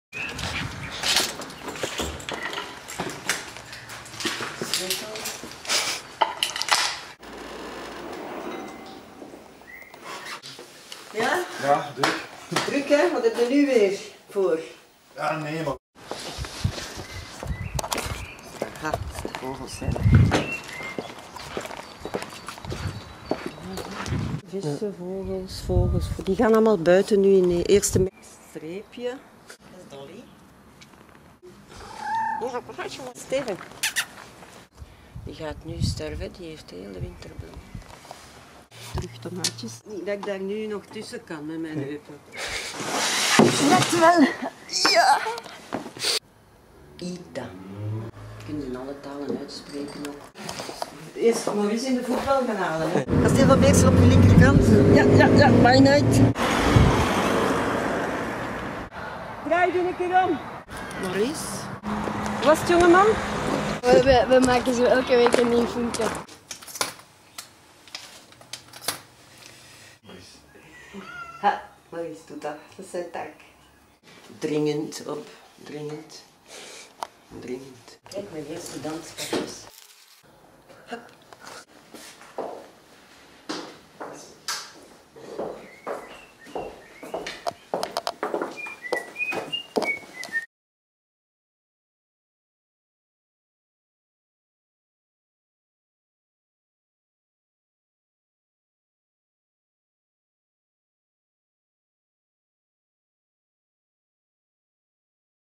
WOMEN-ida
ida de vos

WOMEN a choreography for 8 female dancers

dance: Louise Chardon, Marie De Corte, Ida De Vos, Miryam Garcia Mariblanca, Sayaka Kaiwa, Kayoko Minami, Natascha Pire, Karin Vyncke
choreography: Ugo Dehaes